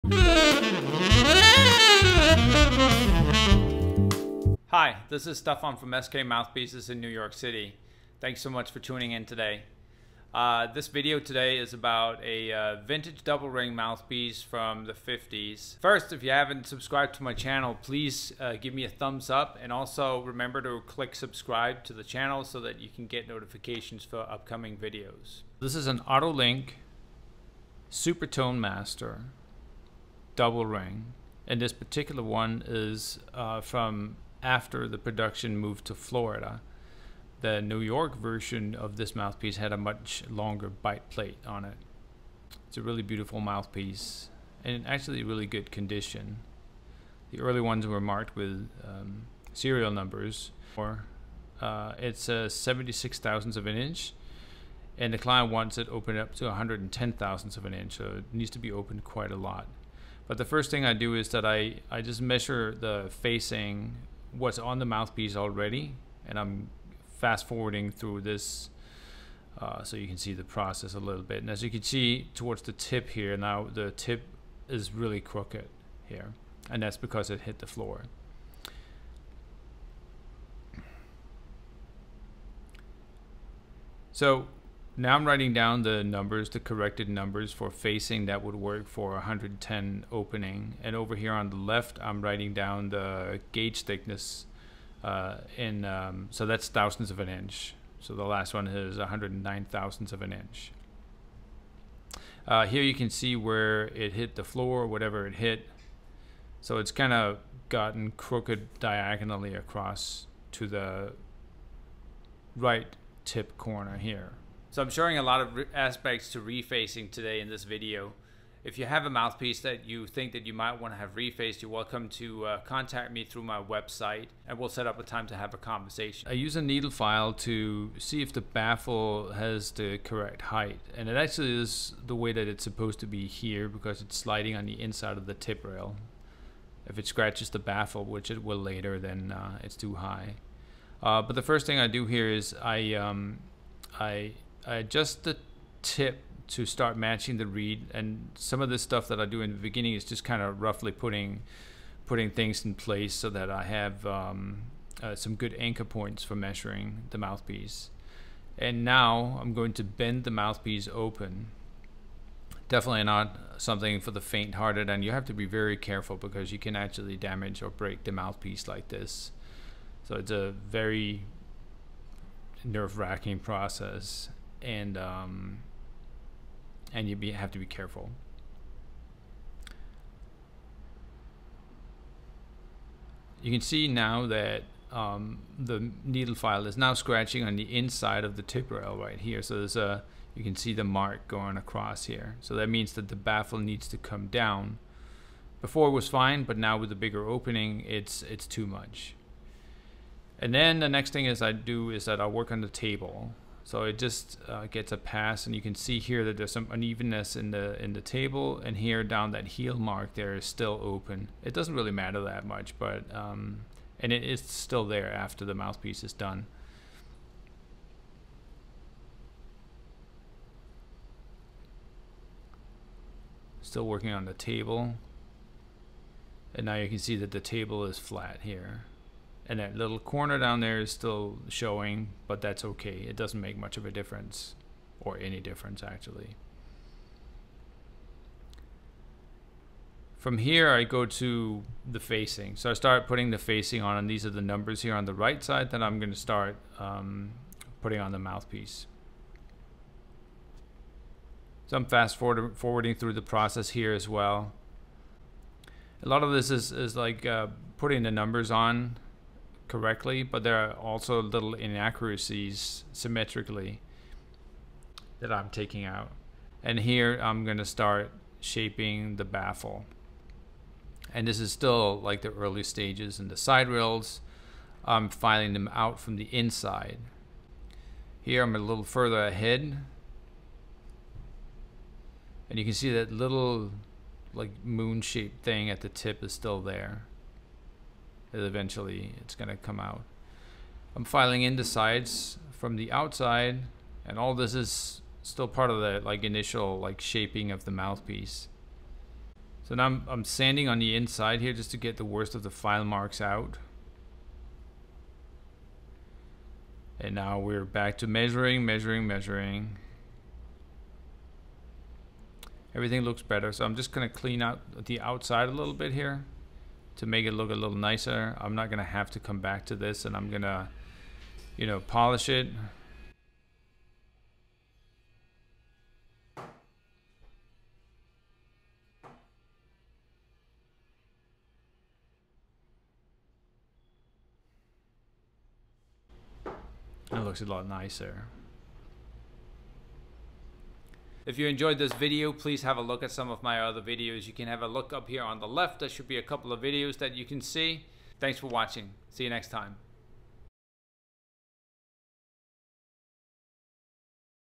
Saxophone Mouthpiece Refacing NYC Opening an Otto Link Double Ring
Some of my clients have stuck with me for many years. Shout-Out To Sam Taylor who has been playing one of my Origin pieces for a while. I completed refacing and opening his latest acquisition, this Florida Double ring. Let’s see which one of them stay on his horn ;-)

EQUIPMENT USED:
Microphone for voice-over
Audio Interface
Lighting Kit